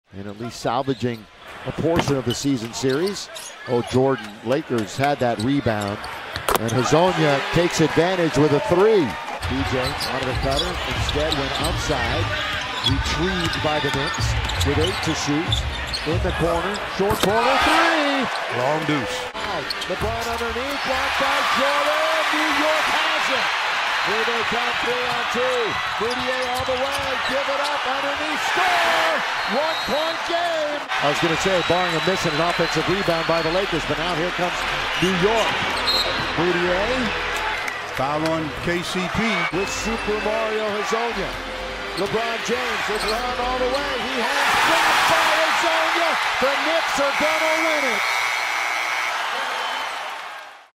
Mario Hezonja Blocks LeBron James' Game-Winner, Stares Him Down In Comeback Win
Super Mario came up CLUTCH.